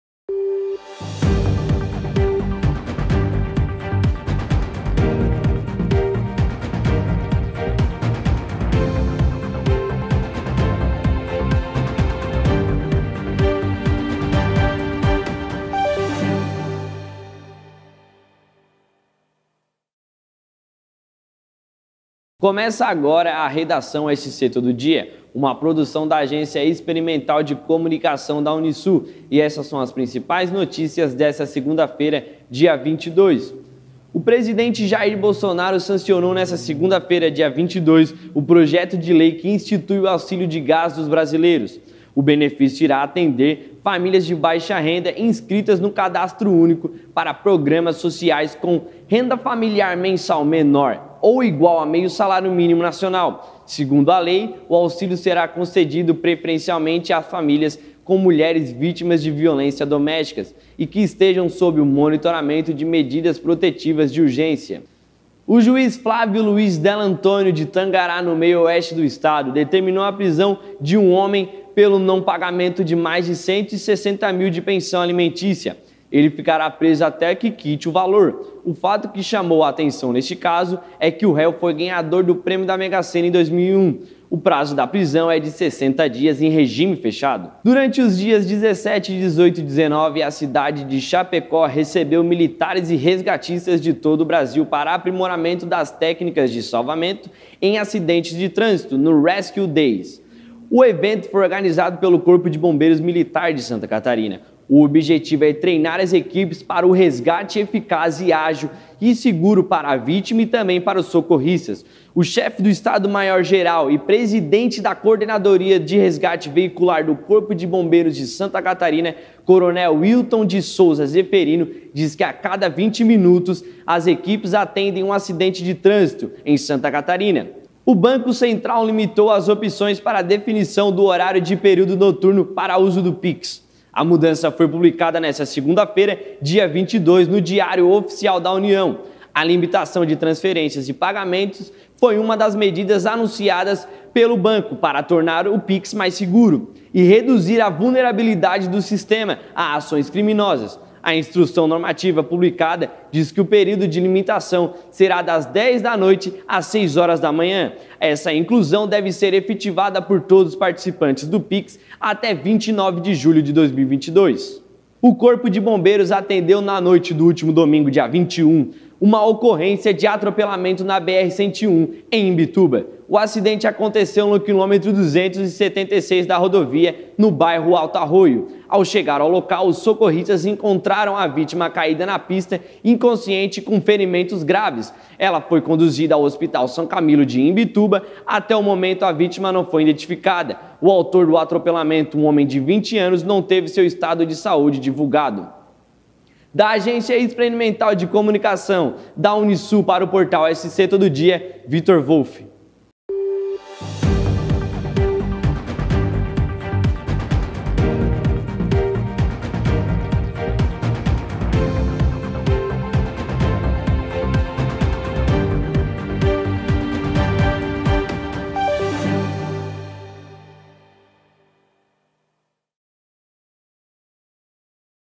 SCTODODIA - NOTÍCIAS DO DIA 22/11/2021
Olá!

A Redação SCTODODIA traz as notícias dessa segunda feira dia 22, uma produção com a Agência Experimental de Comunicação da Unisul de Tubarão e com a apresentação de Vitor Wolff!